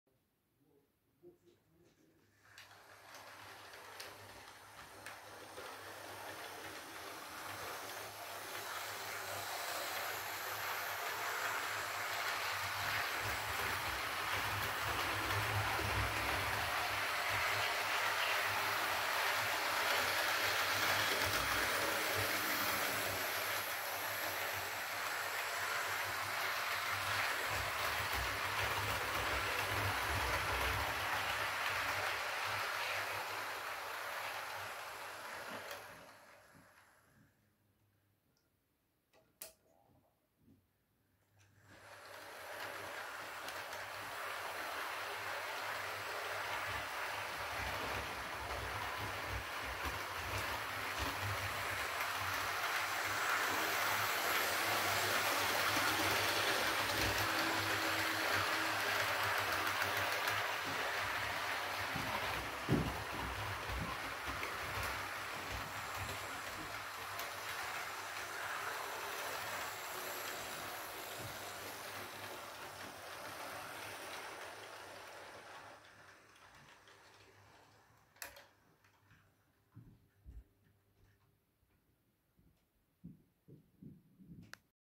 Britannia Pacific Models Class 312 EMU in British Railways Blue & Grey Livery.
This unit is one of the four members of the 312/2 sub class for use around Birmingham.